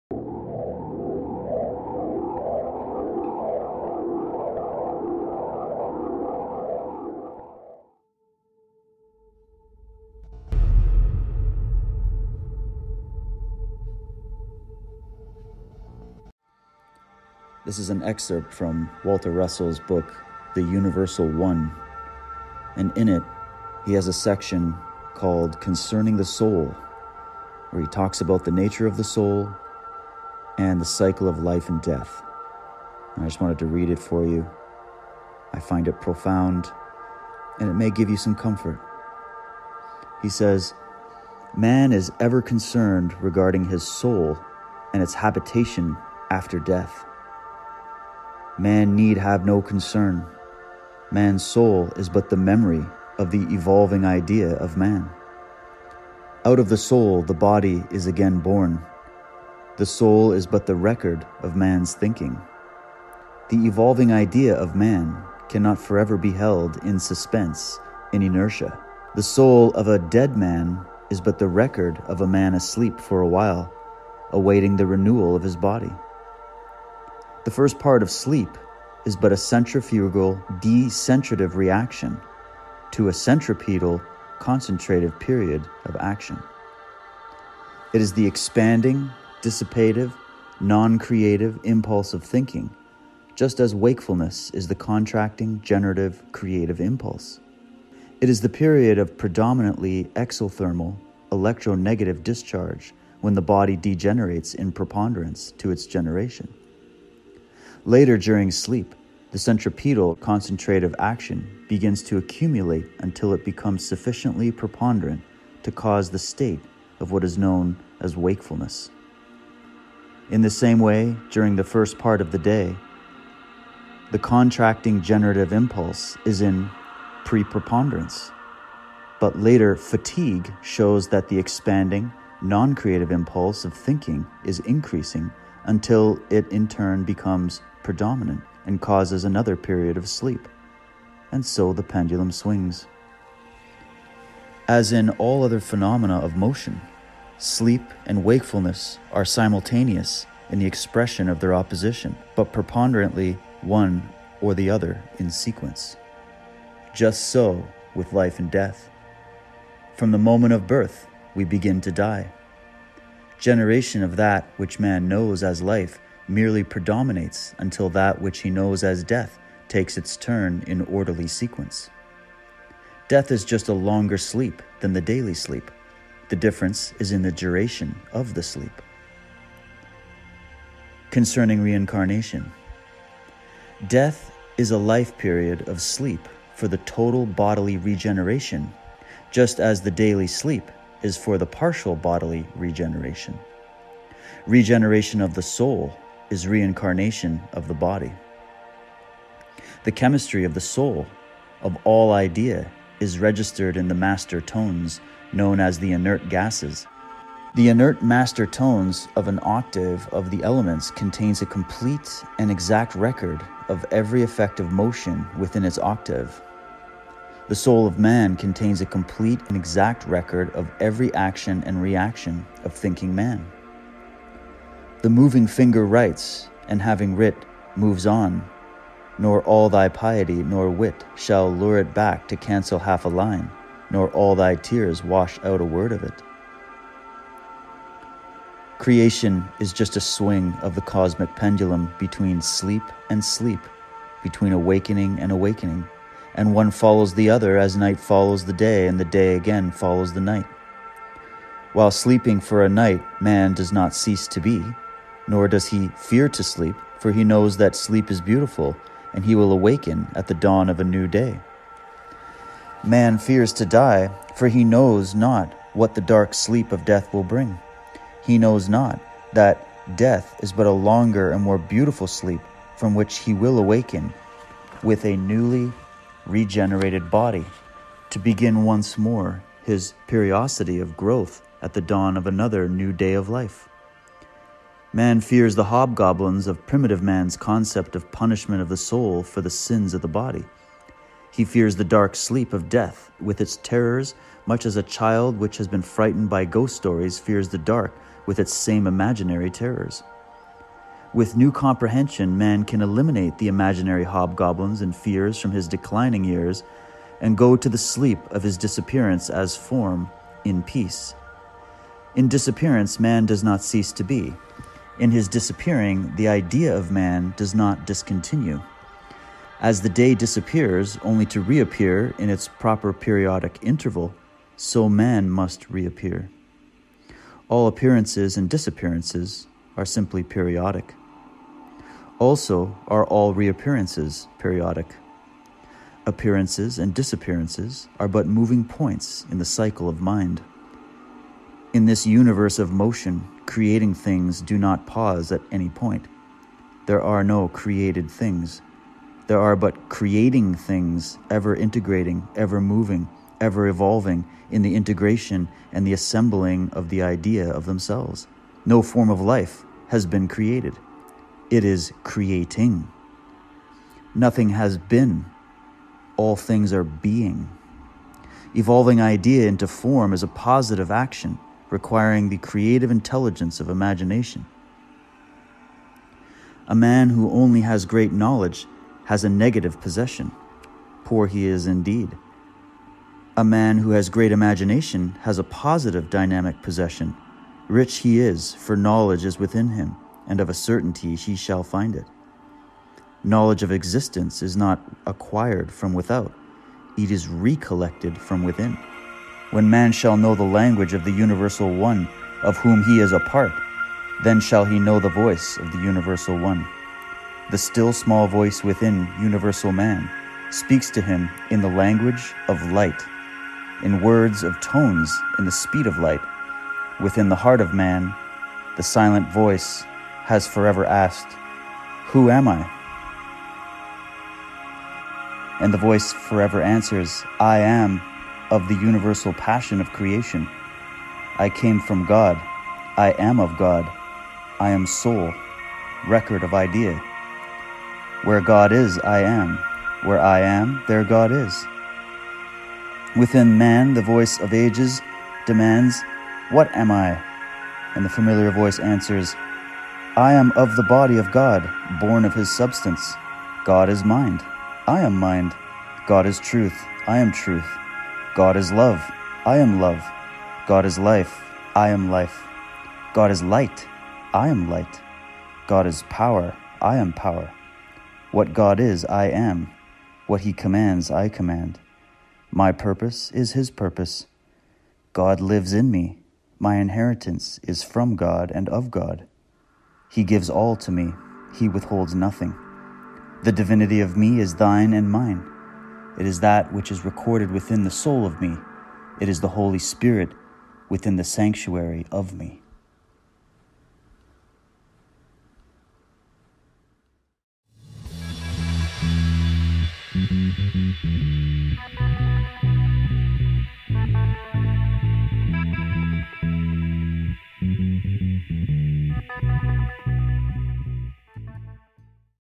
"THERE IS NO DEATH" - Walter Russell
David Whitehead reading from 'The Universal One', by Walter Russell.